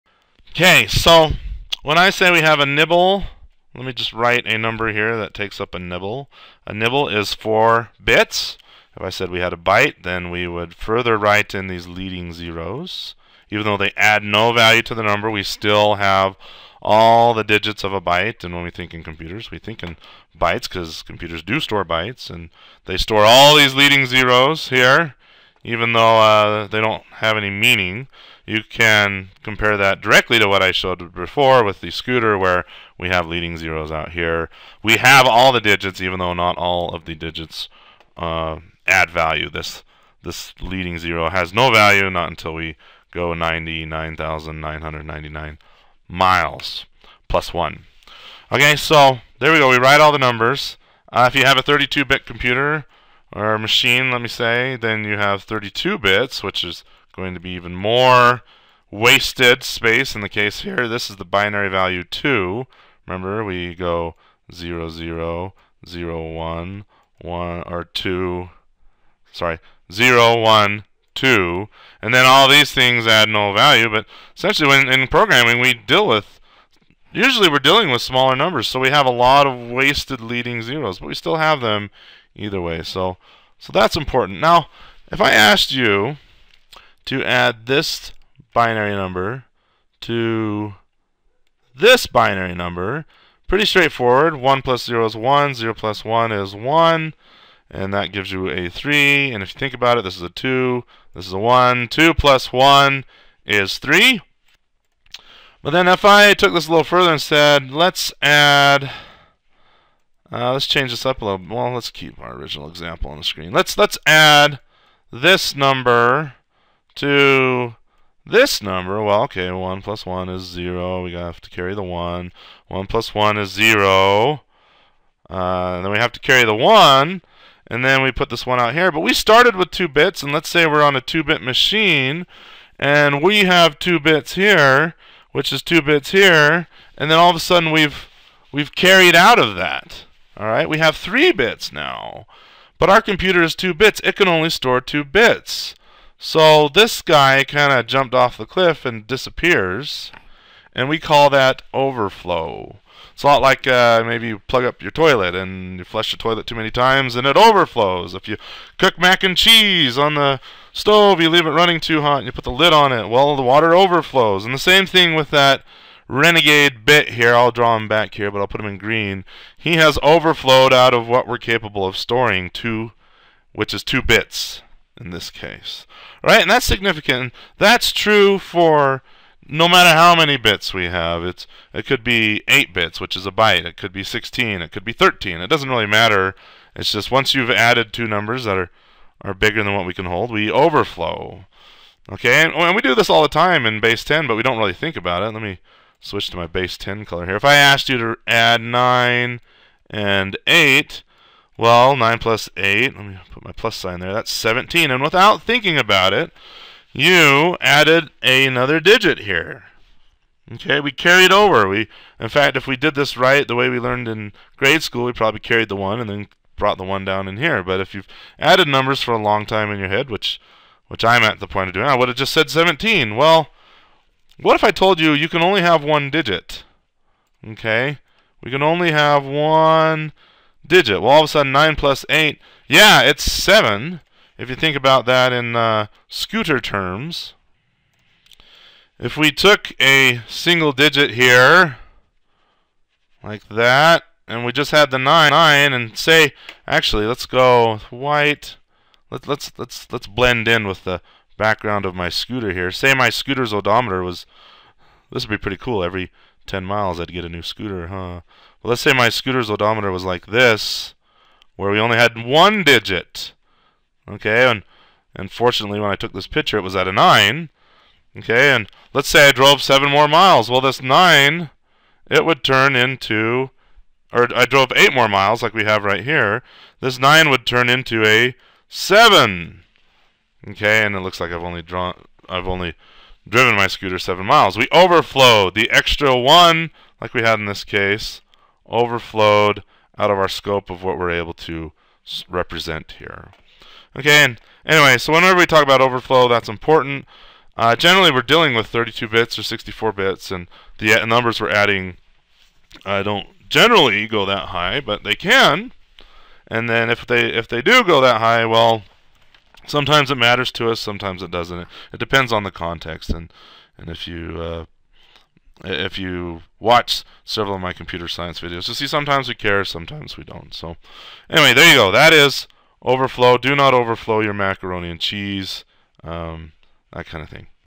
Binary Overflow
Jamie King talking about overflow, what it is, and what causes it.